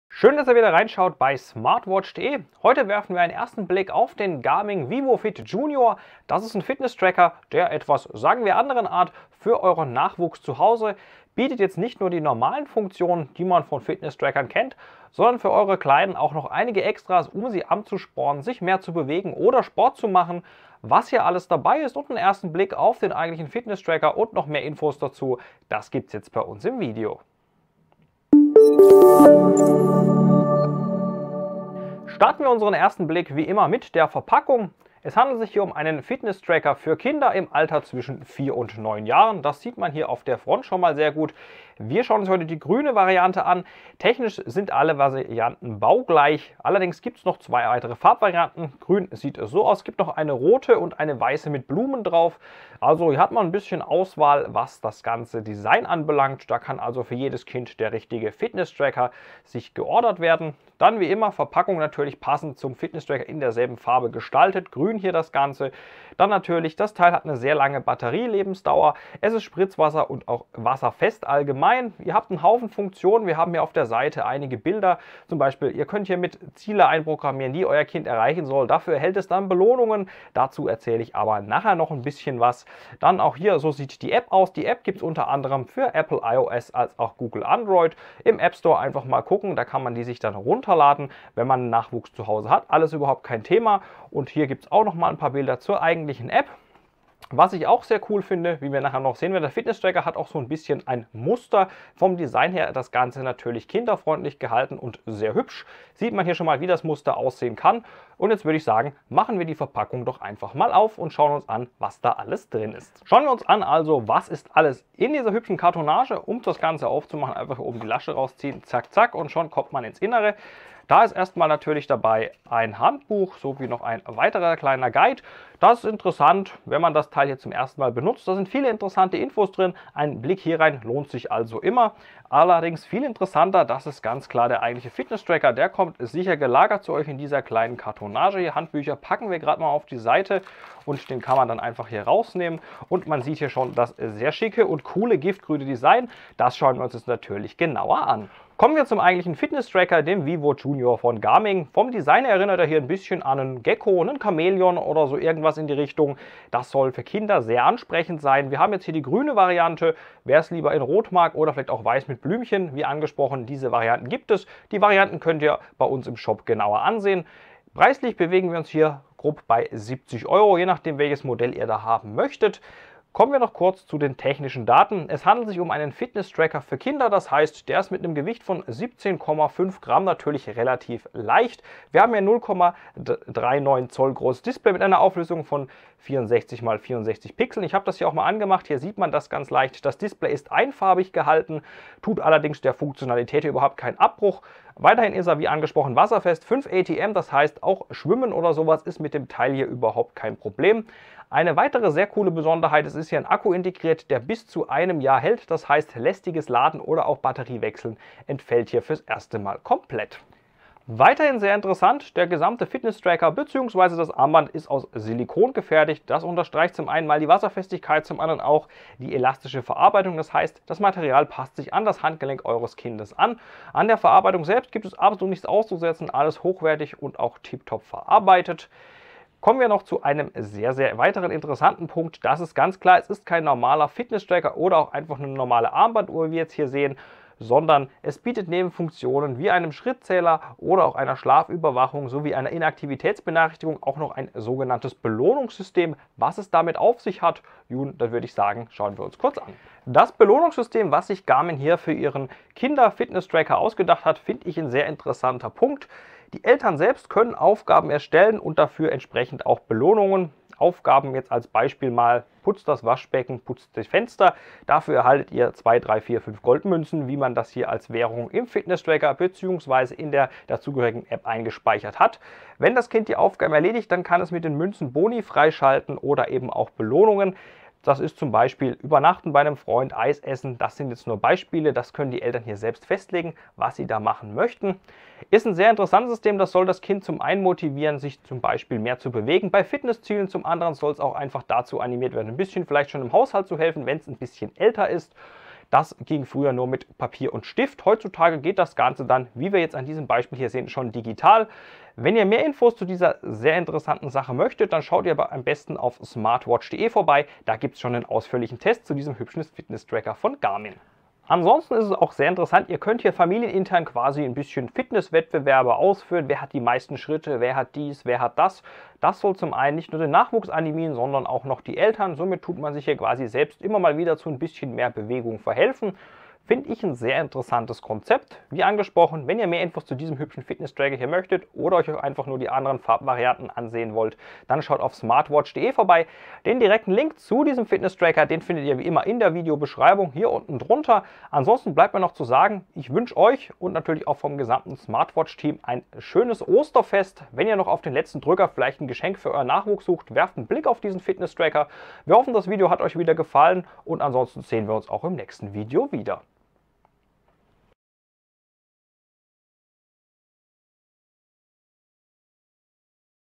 Garmin vivofit jr. - Smartwatch.de Unboxing [DEUTSCH]
Heute beim uns im Unboxing - Die Garmin vivofit jr.

Mit dem Garmin vívofit jr. bringt der Hersteller erstmals einen Fitness-Tracker für Kinder auf den Markt. Das wasserdichte Wearable ist speziell für Kinder im Alter zwischen 4-10 Jahren entwickelt und erfasst deren tägliche Aktivitäten. Eltern können für ihre Sprösslinge Aufgaben, aber auch Belohnungen für erreichte Ziele definieren. Auf der anderen Seite können Kinder mit Hilfe der App motivierende, lustige Abenteuer erleben und sich sportlichen Herausforderungen stellen.